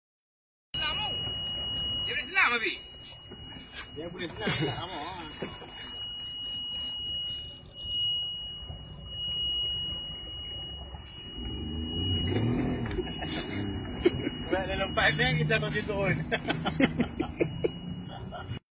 [VIRAL Erk] Dugung ke Babi di tengah laut tu
Sekali Jumpa babi di tengah laut.rupa-rupanya babi berkeupayaan berenang merentasi pulau ke pulau